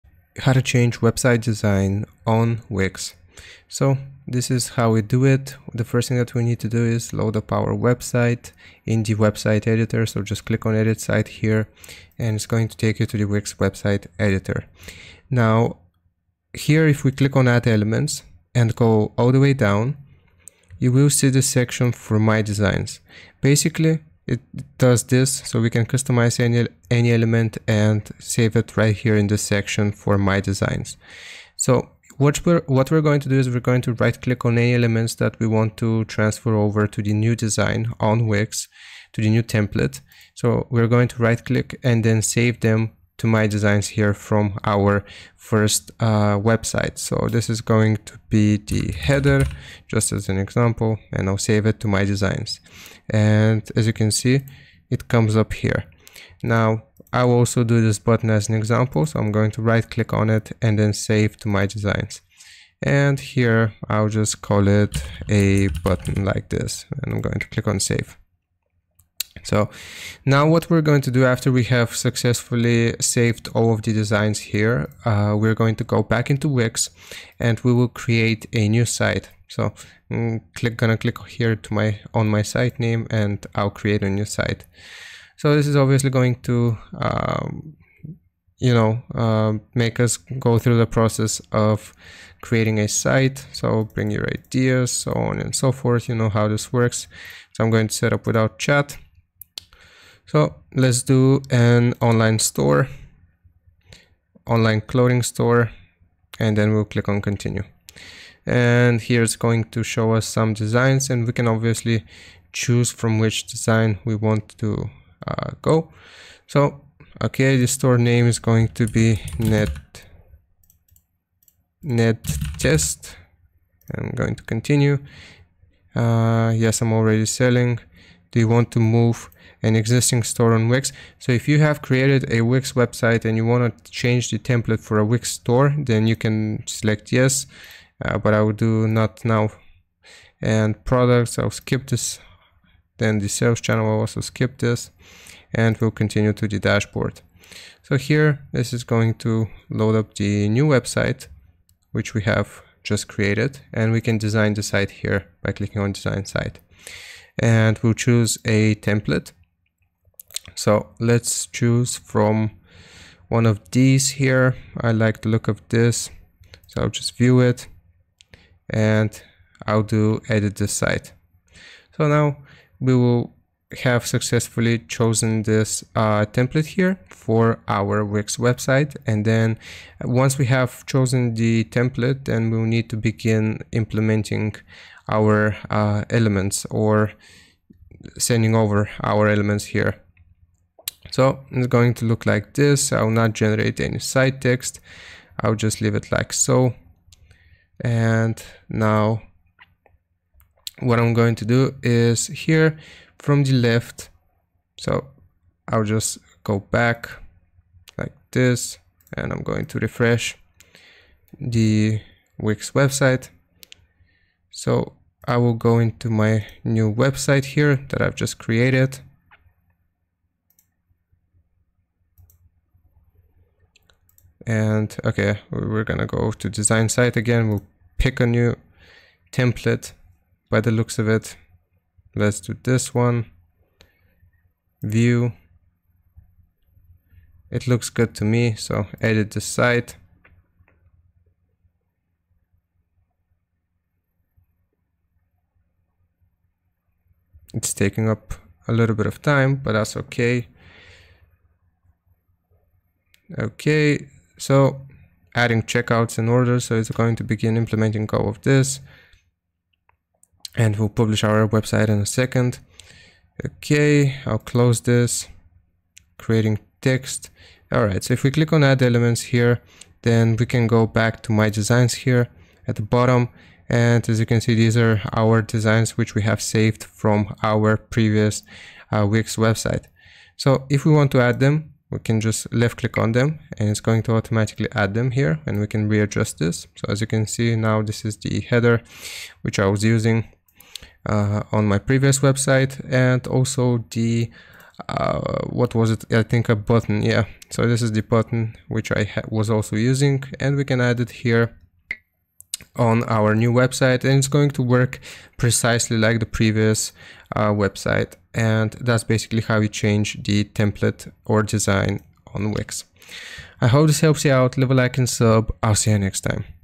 How To Change Website Design On Wix 2025
In this video I show you how to change your website design on Wix in 2025. If you want to learn how to change template for an already existing Wix site then this is the tutorial for you.